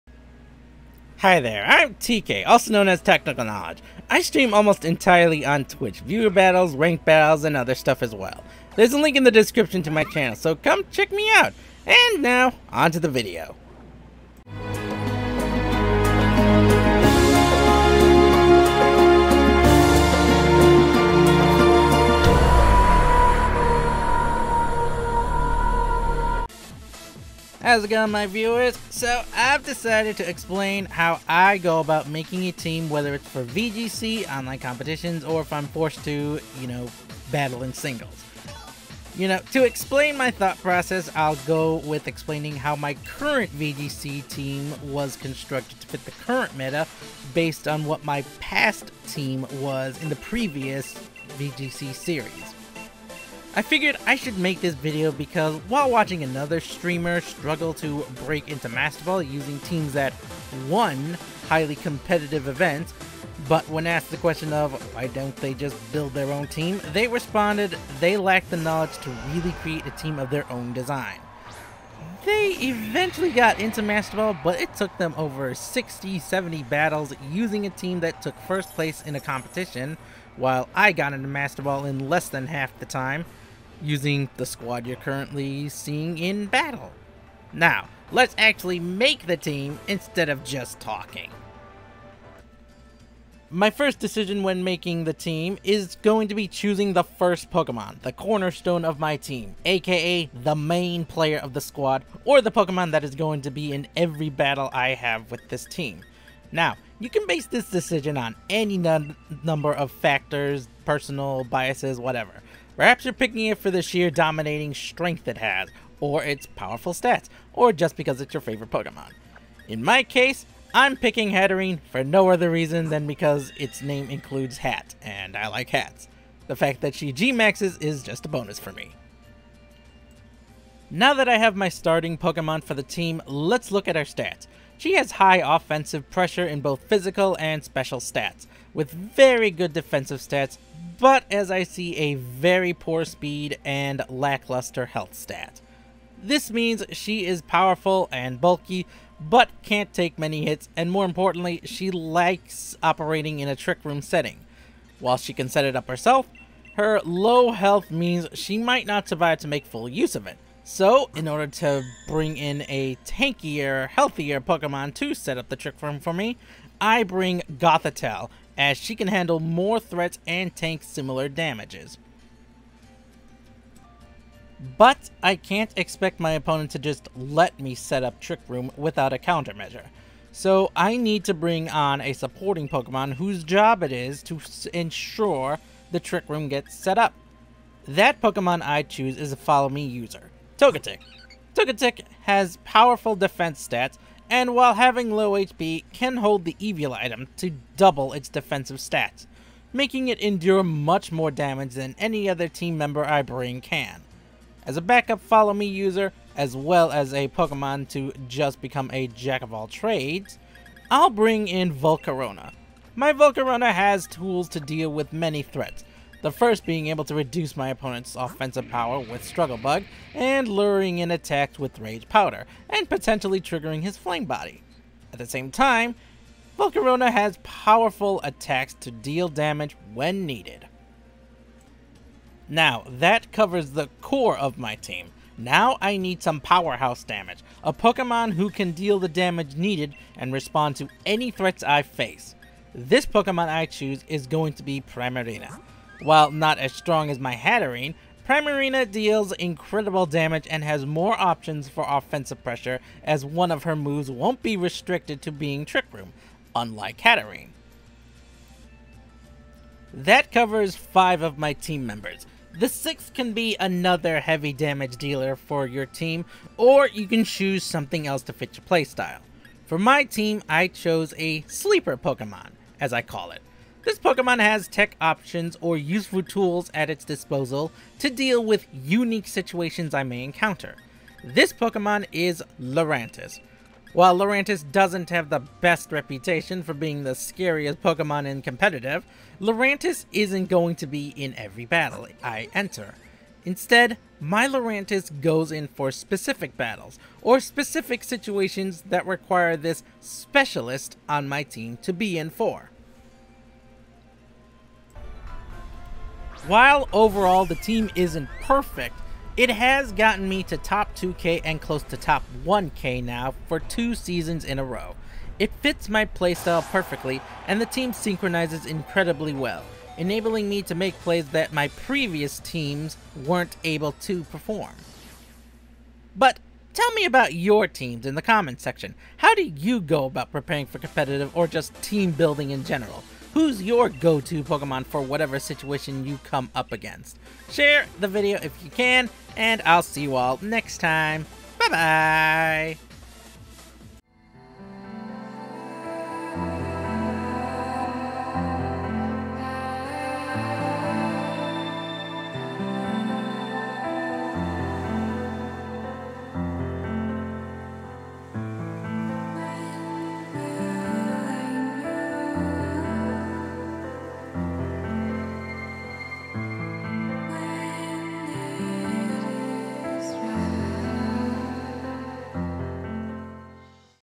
How TK Builds His VGC Teams
Building a VGC Team, especially a competitive one can be very hard to achieve for any trainer, how do they take the pieces of a massive puzzle and fit them together to form a smaller picture that looks like it works?

The answer to that is simple, follow the Reshiram as he shows you how he made Team Hat and his thought process to 'making' the team!

Reshiram Image used in thumbnail/video courtesy of official TacticalKnowledge Channel Artist @Ghostless